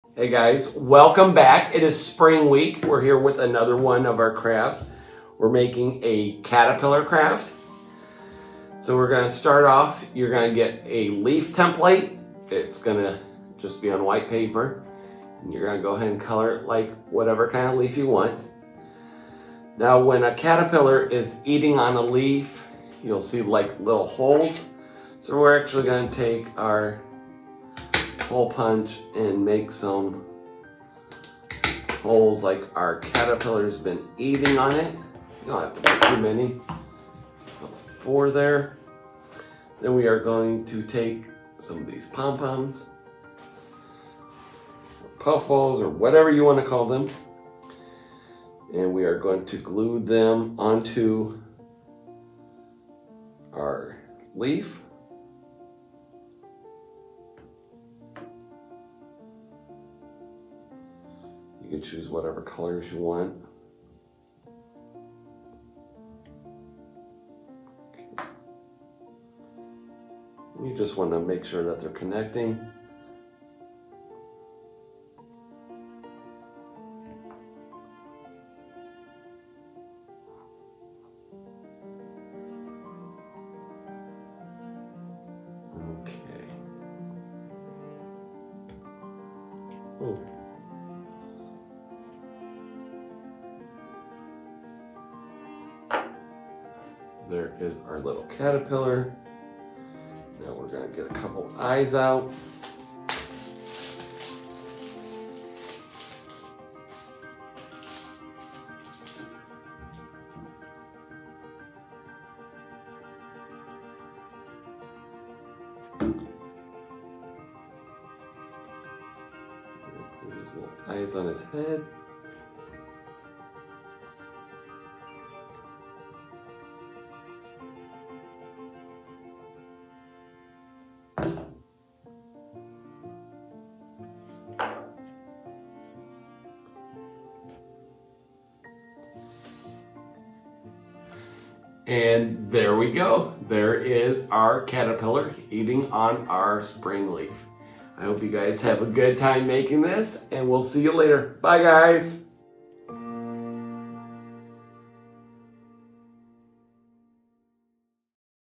DT Crafts: Pom-Pom Caterpillar
Matt is here with a Pom-Pom Caterpillar for Spring Week. It’s cute!